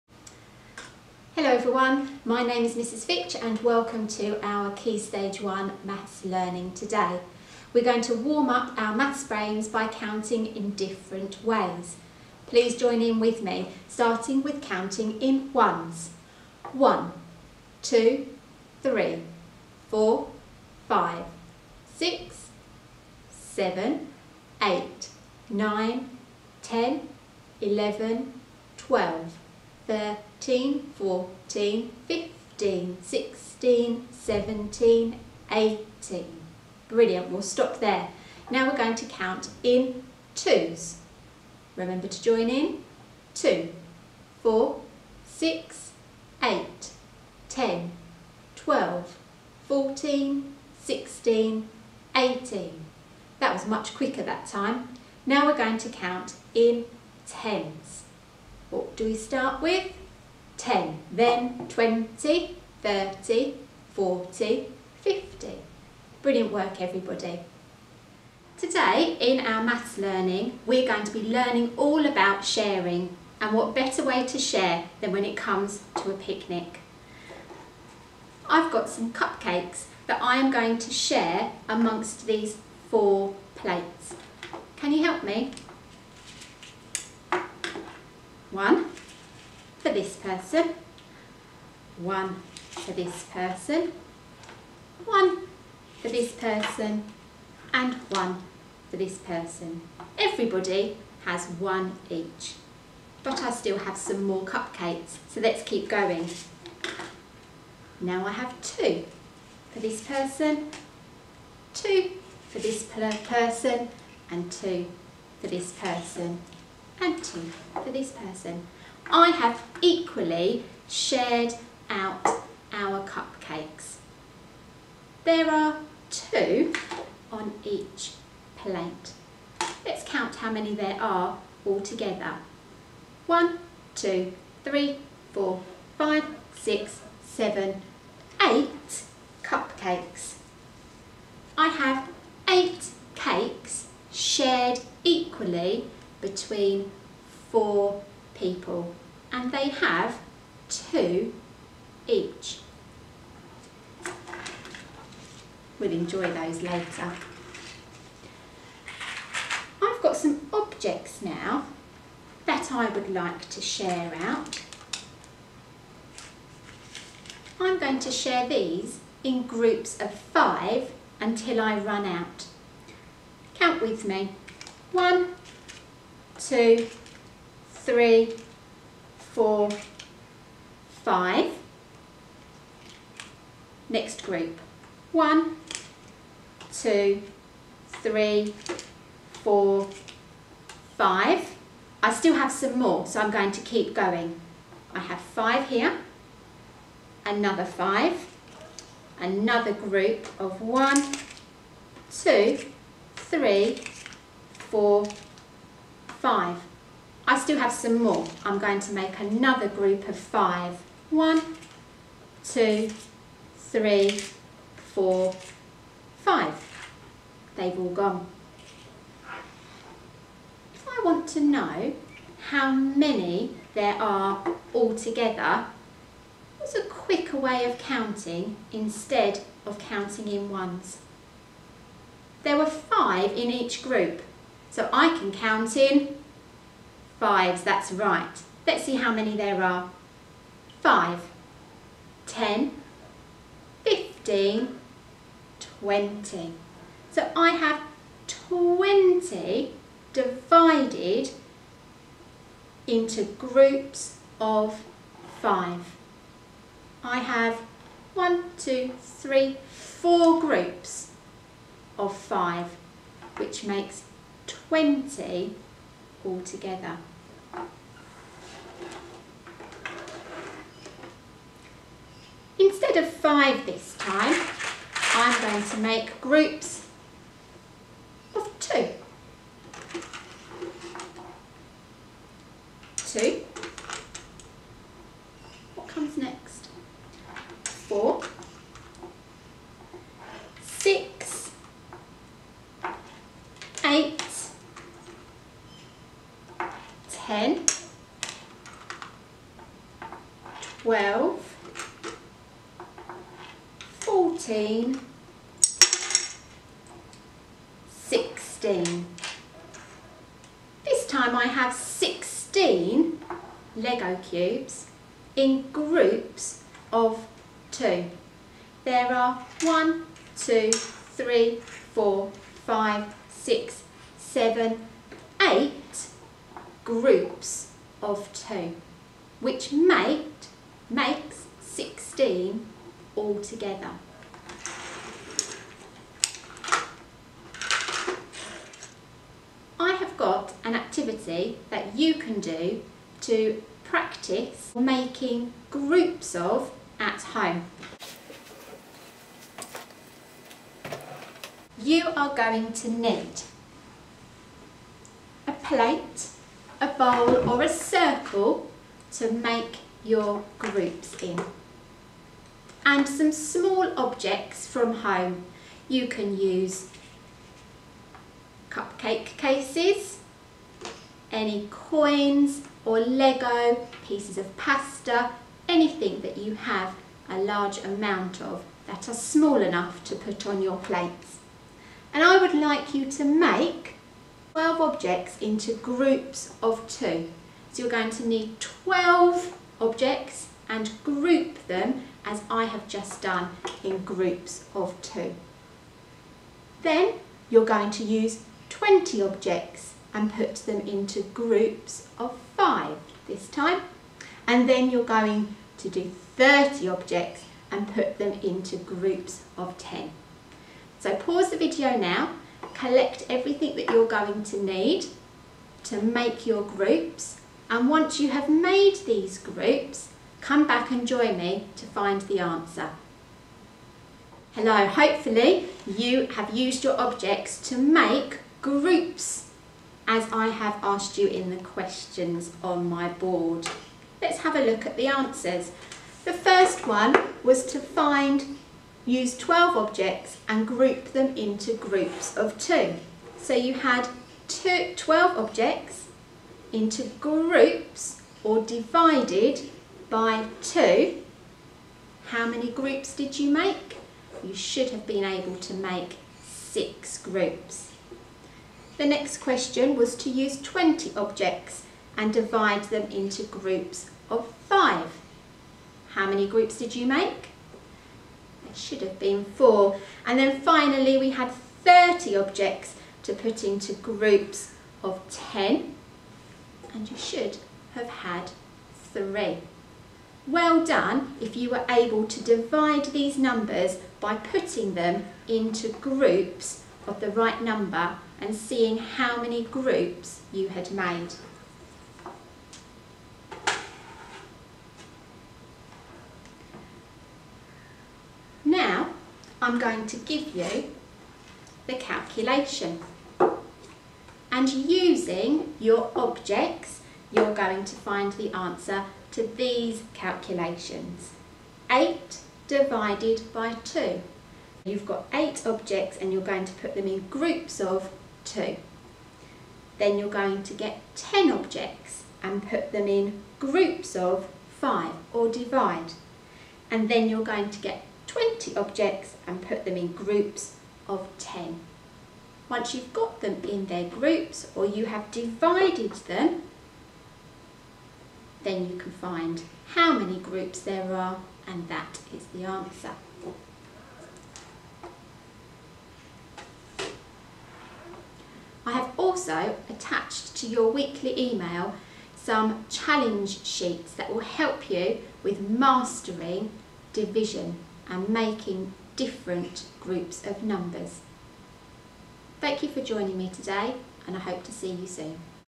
Year 1 Maths - Division
Join Mrs Fitch for the final maths lesson of the term which is all about sharing. Can you find objects in your house that you could share out onto plates or into piles? After the lesson, keep practising sharing and then writing division number sentences.